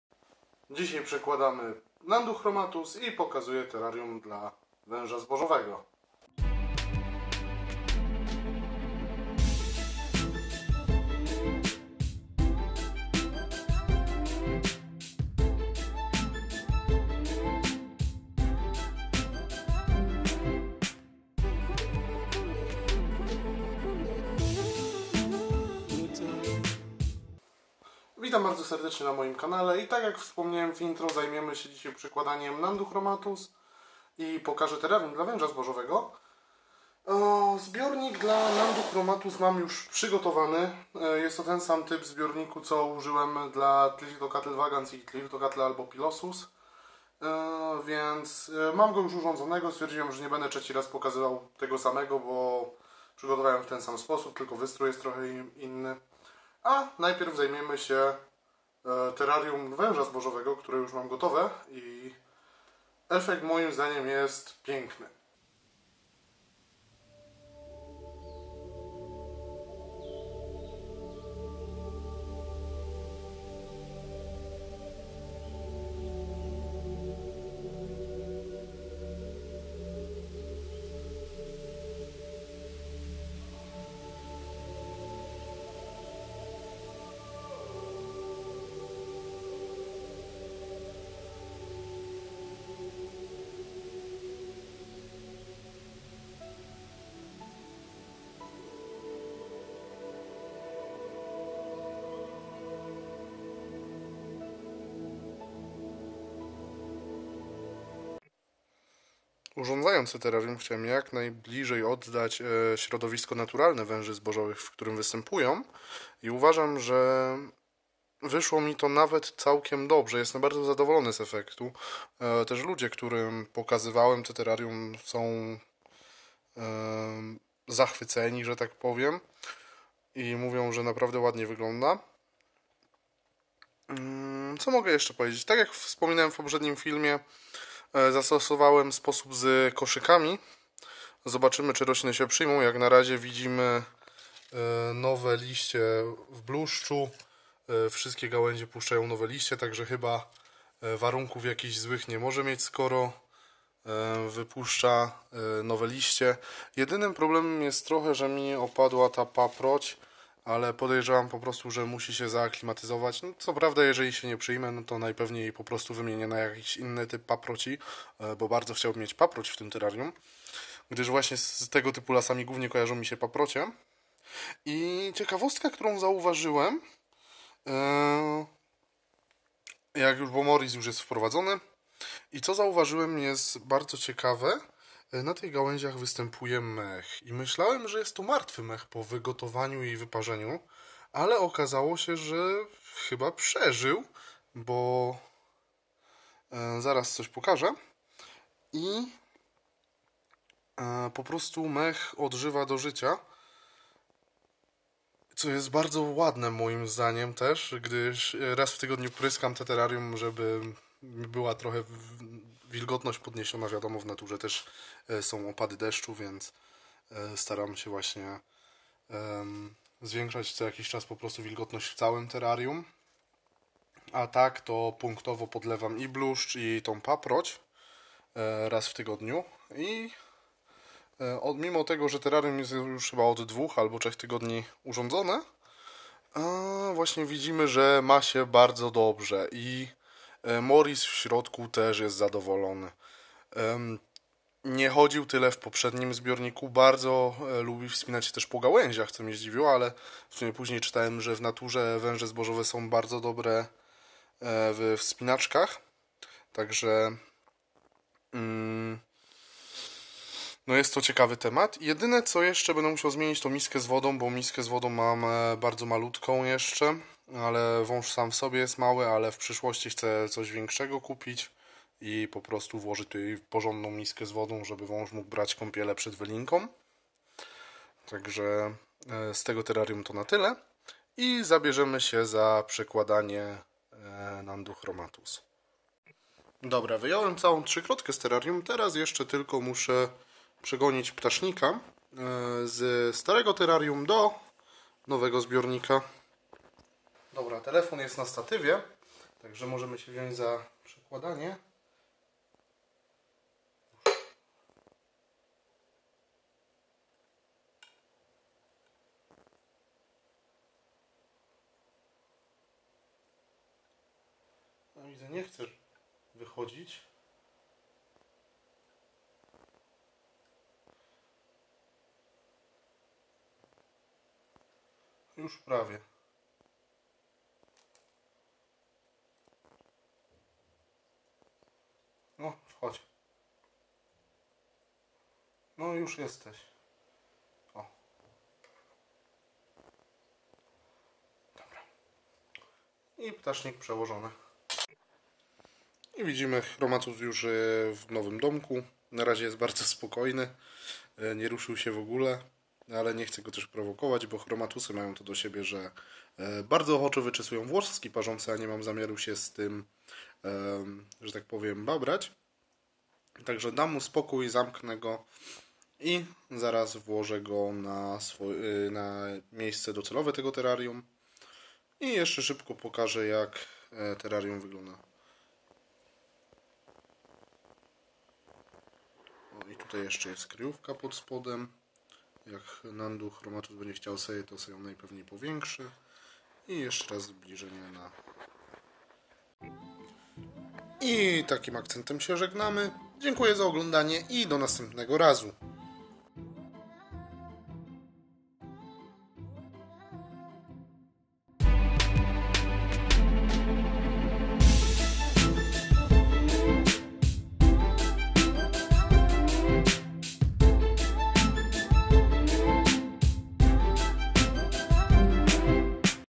Terrarium dla Węża Zbożowego gotowe + Przekładamy Nhandu chromatus
W tym nieco krótszym filmie pokazuję urządzone terrarium dla weża zbożowego oraz przekładam N. chromatus do nowego zbiornika